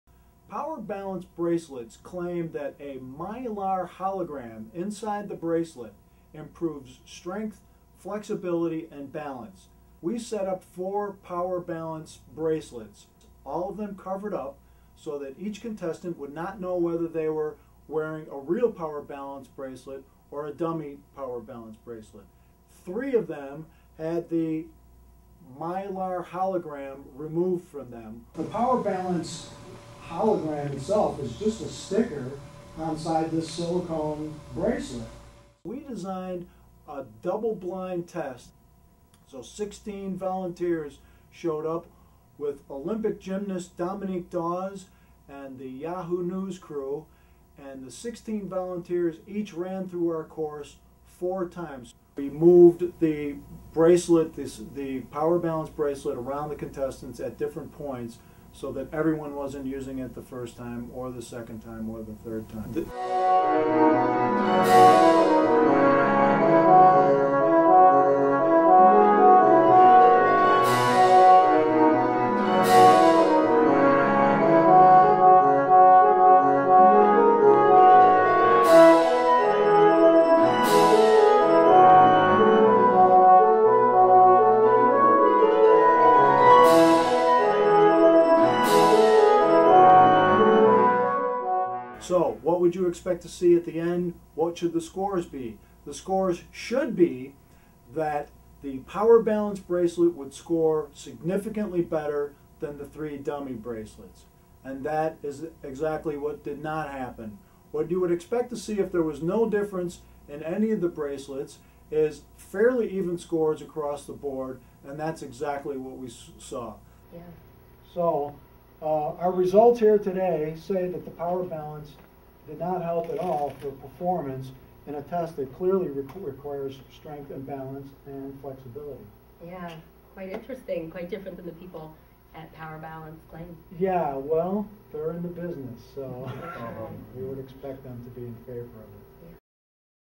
Power Balance Bracelet
Can a bracelet with a hologram make you stronger, more flexible and steadier on your feet? Here's an example of testing a claim using the scientific method.